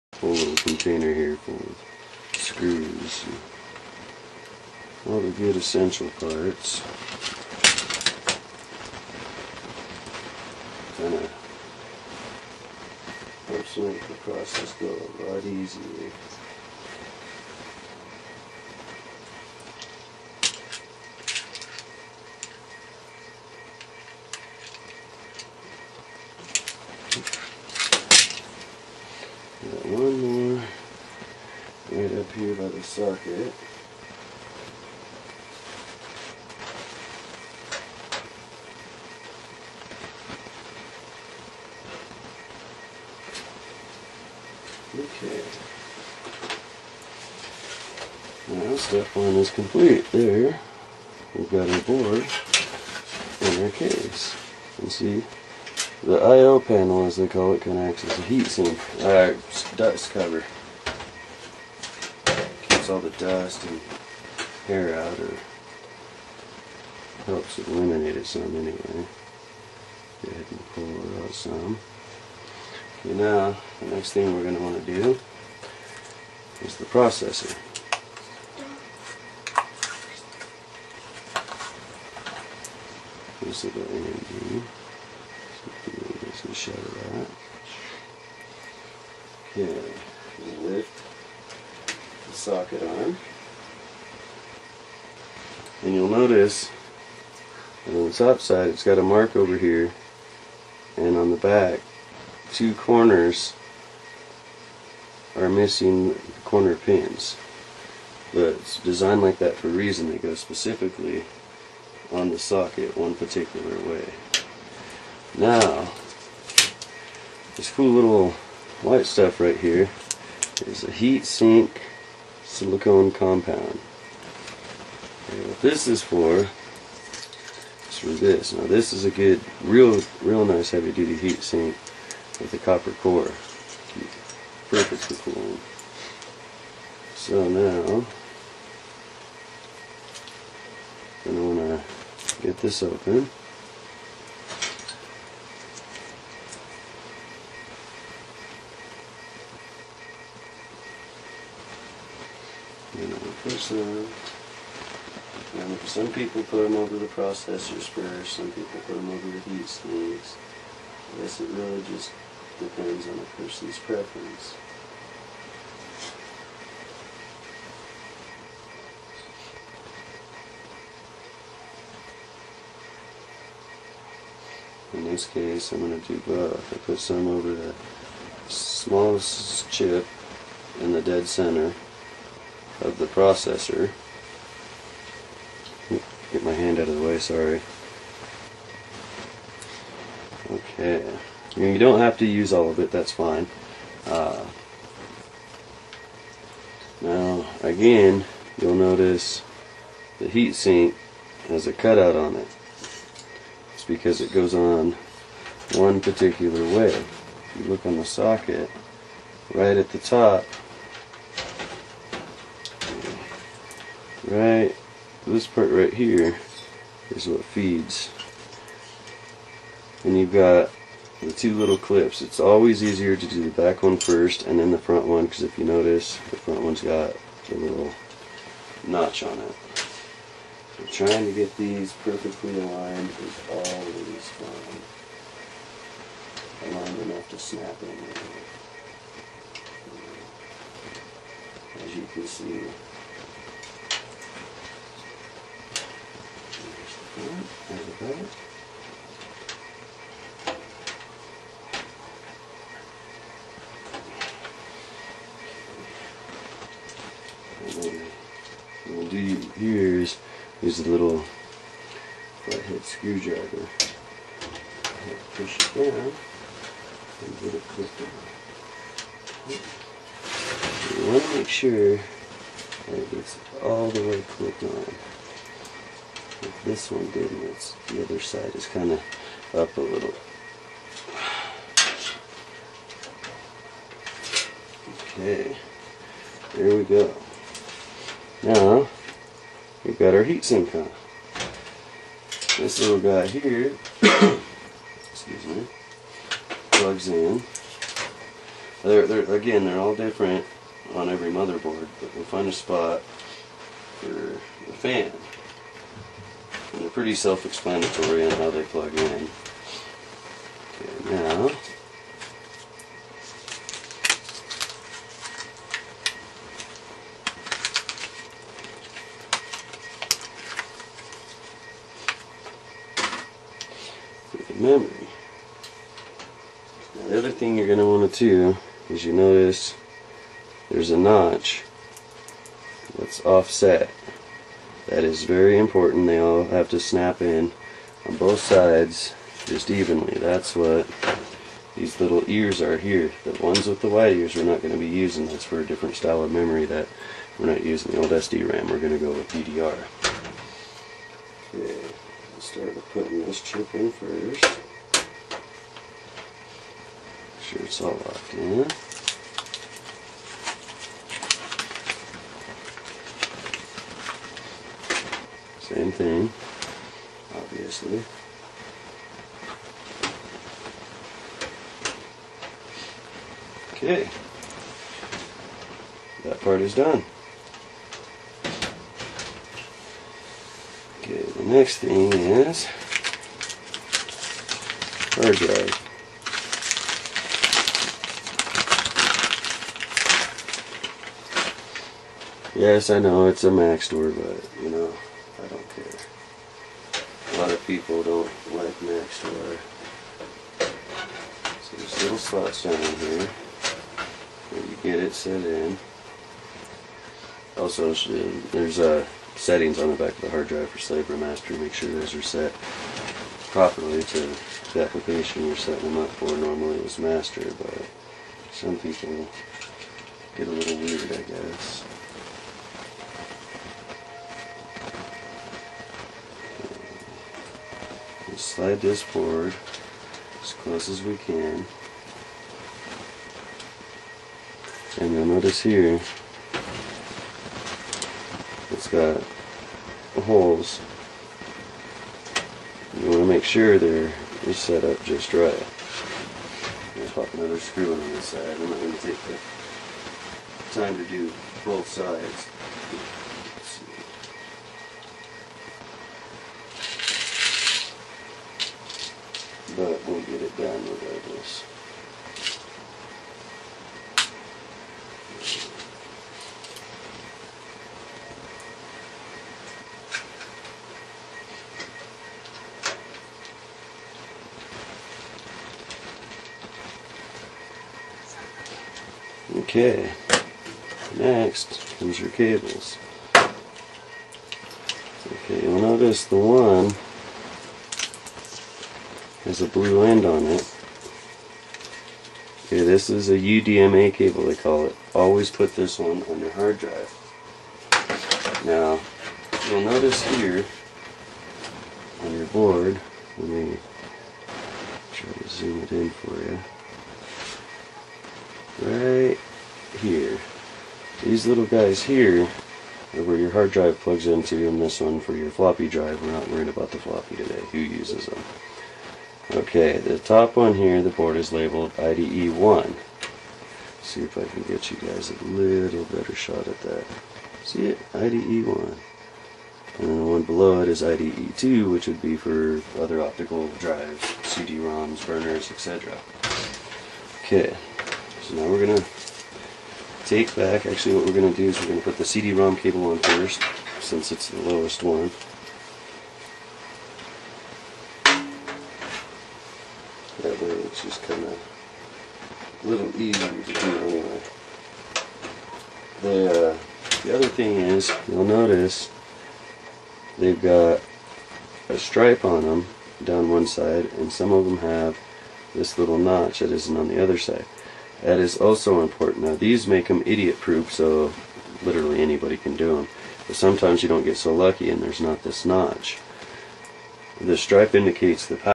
How To Build A Computer from Scratch!! Part 2,Continued
Part 2 of 5 on building a computer from scratch.